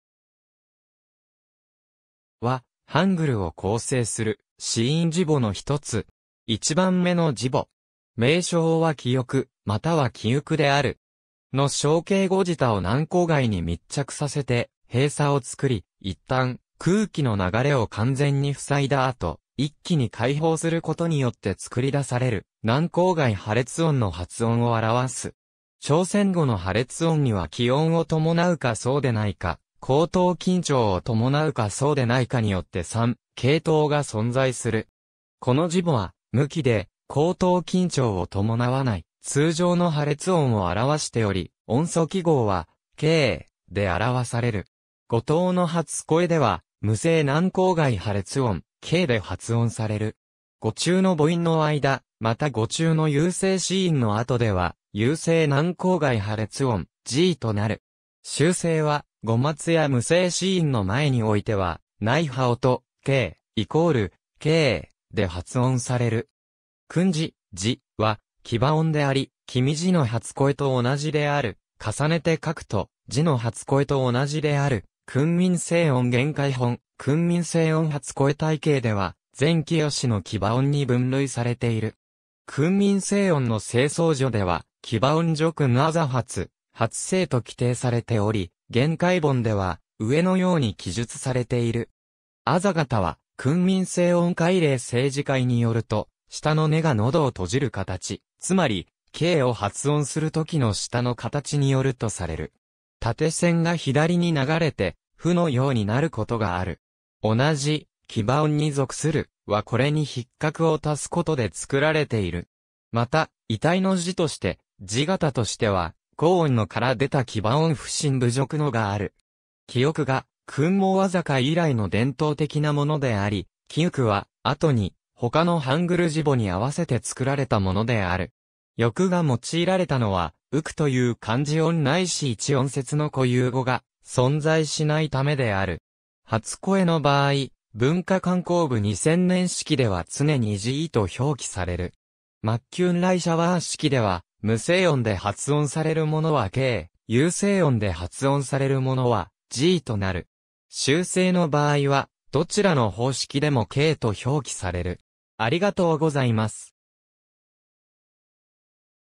ㄱ
ㄱは、ハングルを構成する子音字母のひとつ。
1番目の字母。
名称はキヨク（기역、大韓民国）またはキウク（기윽、北朝鮮）である。
ㄱの象形 後舌を軟口蓋に密着させて閉鎖を作り、一旦、空気の流れを完全に塞いだ後、一気に開放することによって作り出される軟口蓋破裂音の発音を表す。
朝鮮語の破裂音には気音を伴うかそうでないか、喉頭緊張（テンス）を伴うかそうでないかによって三系統が存在する。
この字母は無気で喉頭緊張を伴わない通常の破裂音（平音）を表しており、音素記号は/k/で表される。
語頭の初声では無声軟口蓋破裂音[k]で発音される。
語中の母音の間、また語中の有声子音の後では有声軟口蓋破裂音[g]となる。
終声は、語末や無声子音の前においては内破音[k̚] = [k_「] (X-SAMPA)で発音される。
訓民正音の世宗序では「牙音如君字初發聲」と規定されており、諺解本では上のように記述されている。
字形は『訓民正音解例』制字解によると、舌の根が喉を閉じる形、つまり[k] を発音するときの舌の形によるとされる。
縦線が左に流れて「フ」のようになることがある。
同じ牙音に属する「ㅋ」はこれに筆画を足すことで作られている。
また異体の字として、字形としては喉音のㅇから出た牙音不清不濁のㆁがある。
「キヨク（其役）」が『訓蒙字会』（1527年）以来の伝統的なものであり、「キウク」は後に他のハングル字母に合わせて作られたものである。
「ヨク（役）」が用いられたのは、「ウク」という漢字音ないし1音節の固有語が存在しないためである。
初声の場合、文化観光部2000年式では常にgと表記される。
マッキューン＝ライシャワー式では無声音で発音されるものはk、有声音で発音されるものはgとなる。
終声の場合はどちらの方式でもkと表記される。